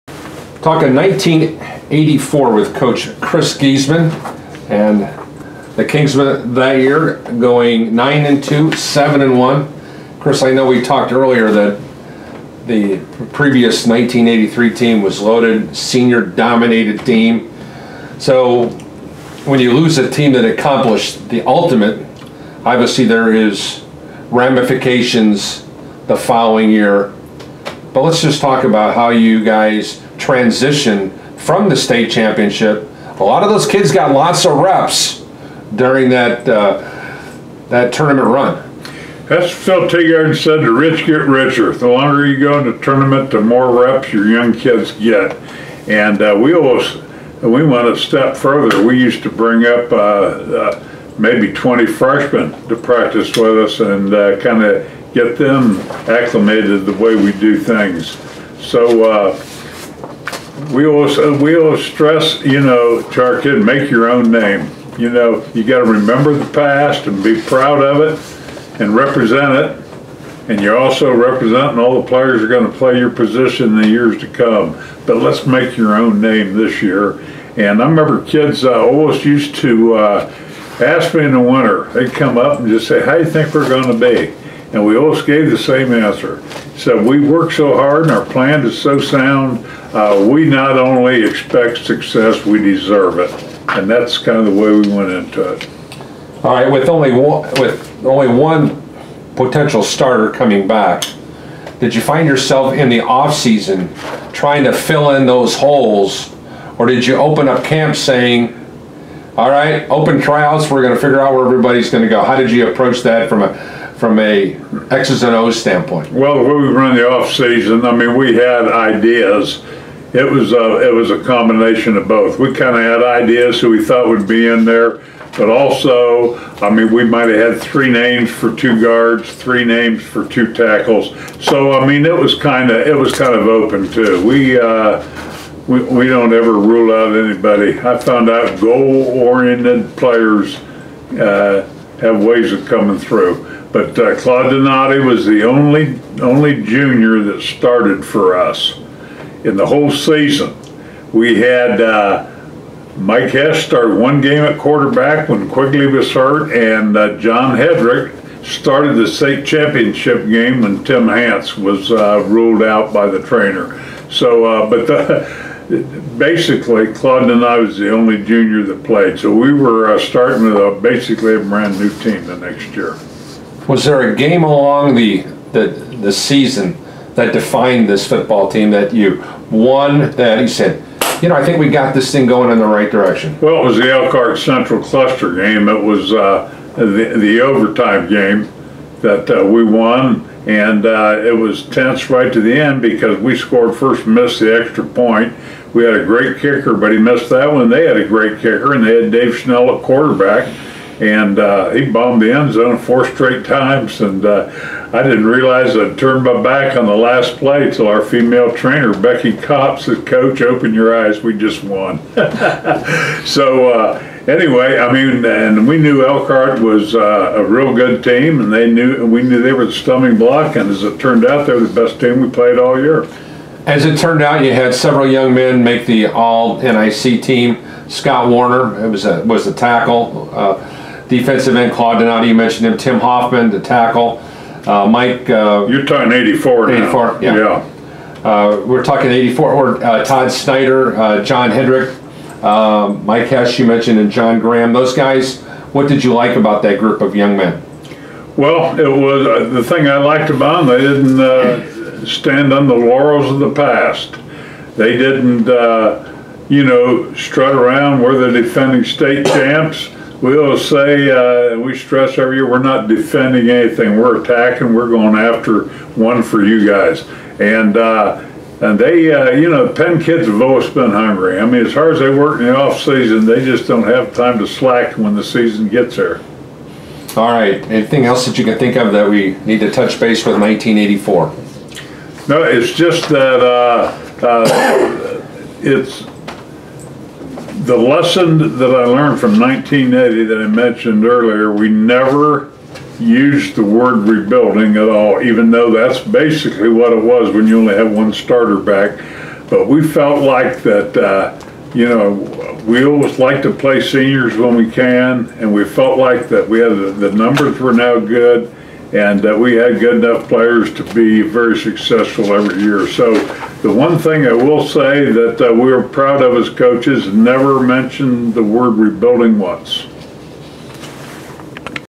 Chris Geesman 1984 Recap for Penn Football Project
Chris Geesman recaps the 1984 season in an unscripted interview for the Penn Football Project